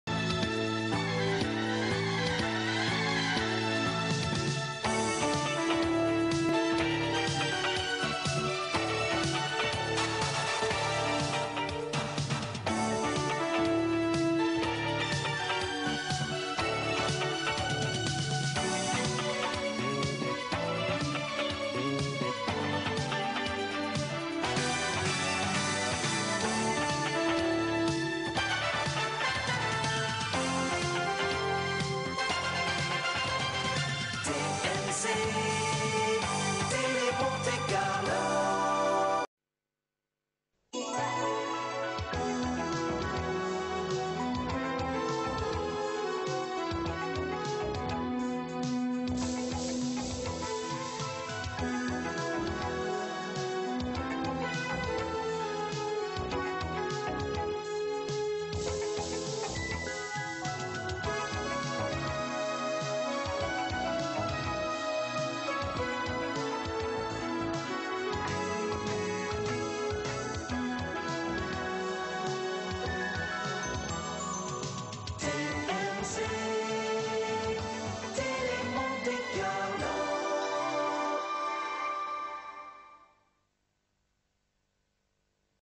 TMC Télé Monte-Carlo - ouverture et fermeture antenne - 1986-1988
Génériques d'ouverture et fermeture d'antenne de TMC (Télé Monte-Carlo) utilisés de 1986 à 1988.